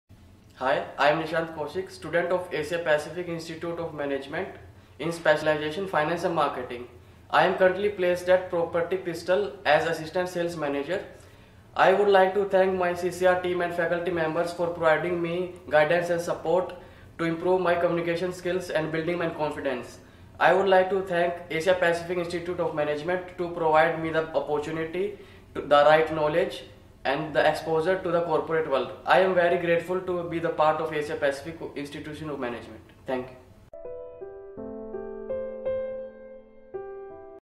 Asia-Pacific Institute of Management | Student Testimonials
Nishant Kaushik, pursuing PGDM in Finance & Marketing at Asia-Pacific Institute of Management, New Delhi is placed at Property Pistol Pvt Ltd. He feels proud to be a part of AIM as he got the opportunity to build her skills and grow in his career.